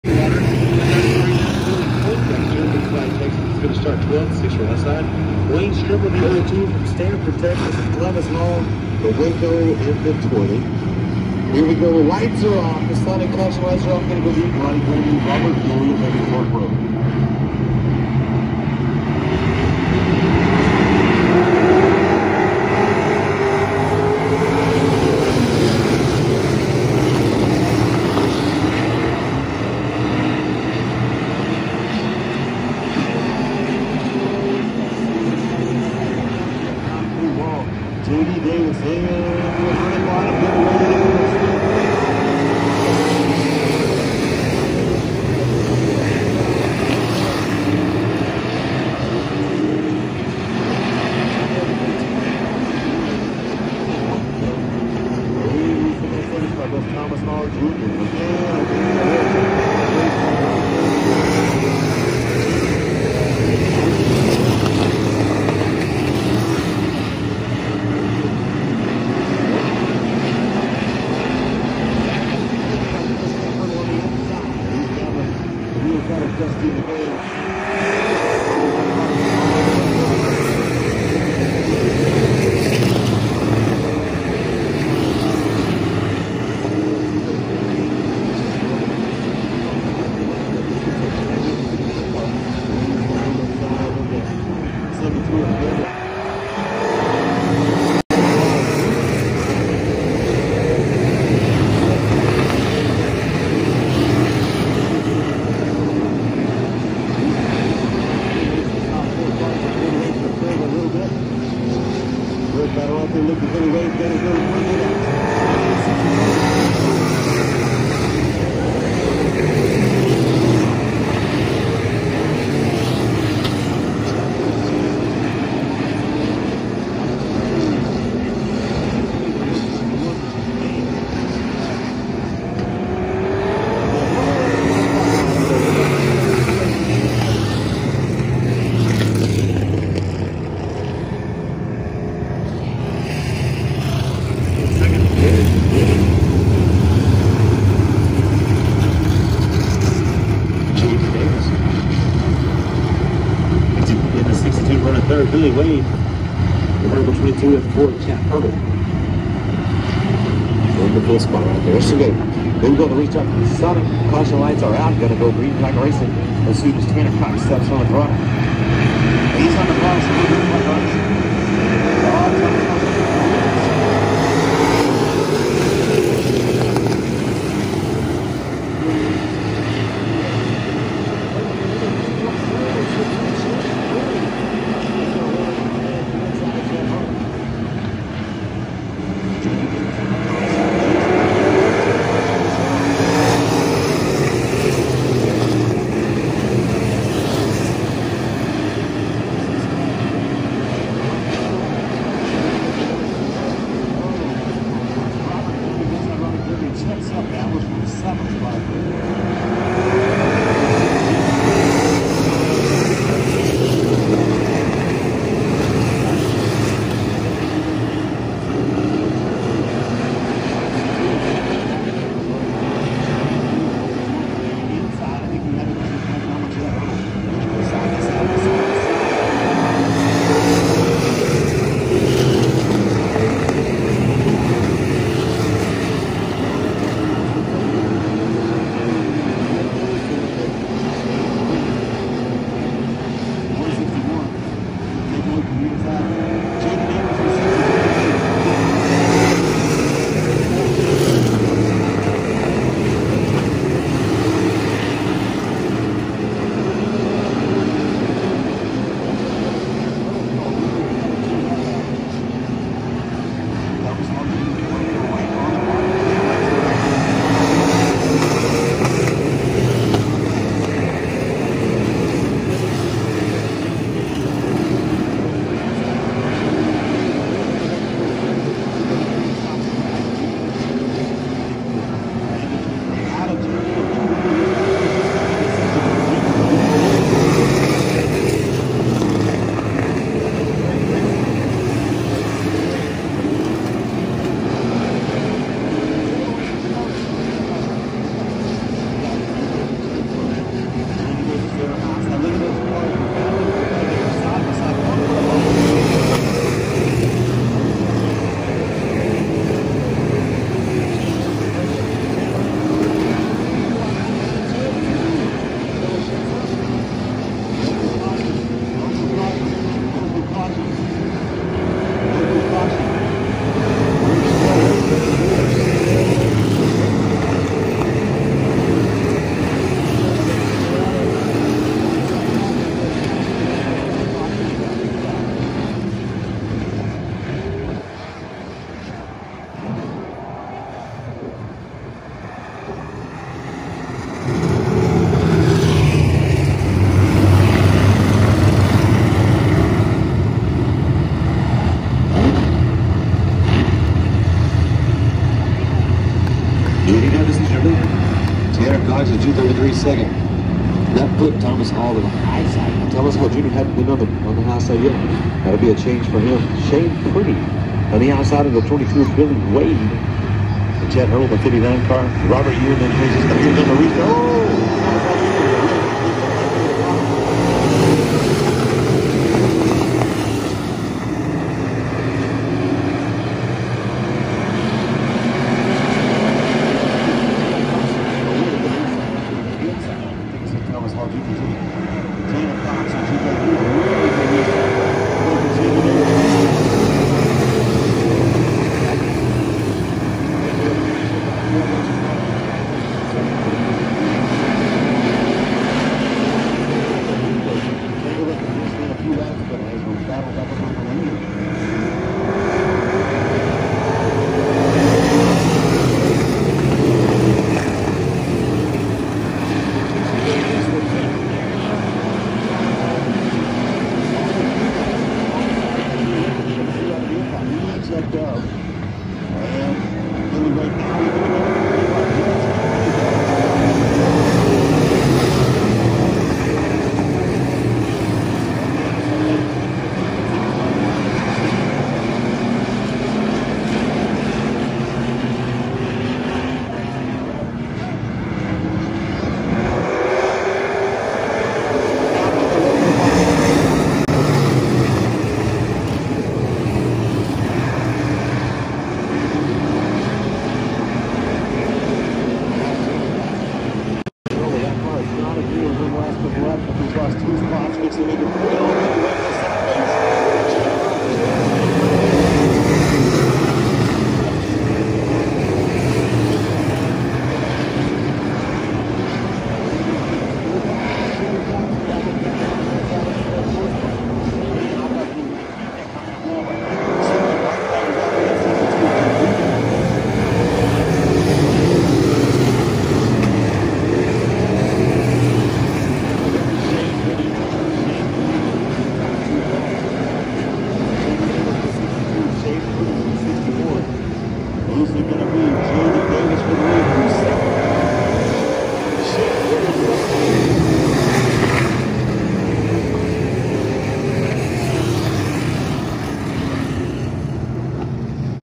Abilene Speedway | 2022 May 21 | Tanner Cox | A Feature (4th)
Abilene Speedway 2022 May 21 Tanner Cox A Feature Saturday IMCA Stock Cars